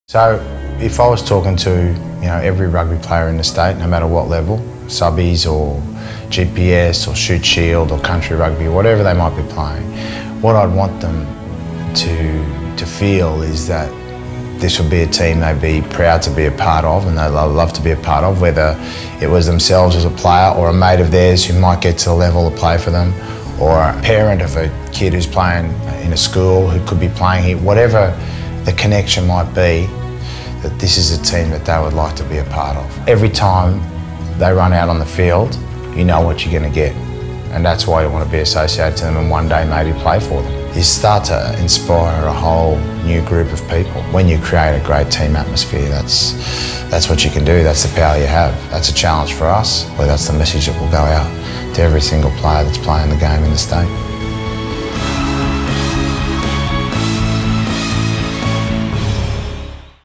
This is a team you'll be proud to be part of: Cheika's message to the NSW Rugby Community
This is a team you'll be proud to be part of. That is head coach Michael Cheika's impassioned pledge to the NSW rugby community.

In an exclusive message to the community, Cheika describes the team's ambition to inspire and unite NSW rugby players and its desire to represent the state with pride.